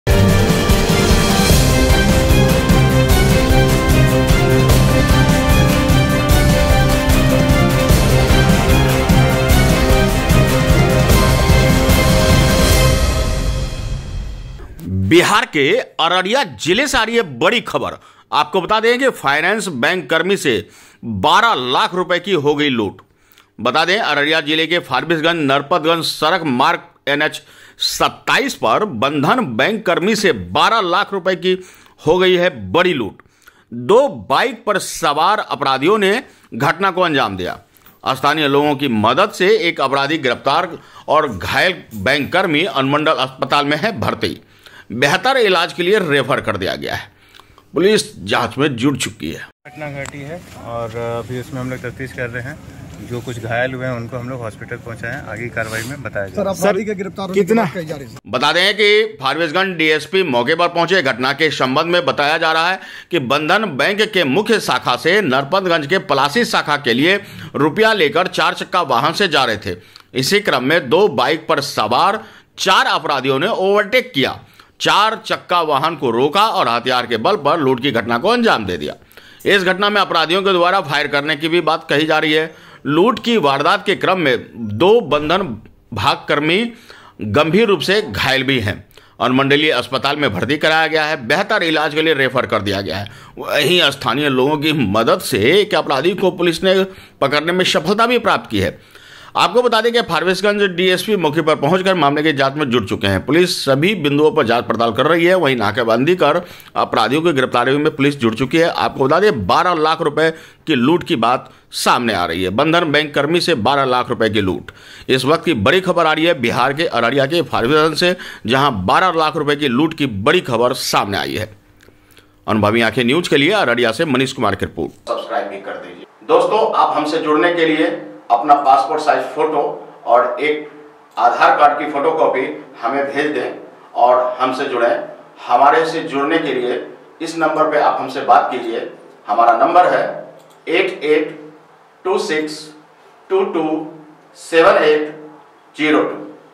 बिहार के सिमांचल में बैंककर्मी से भयंकर लूट,पुलिस के लिए चुनौती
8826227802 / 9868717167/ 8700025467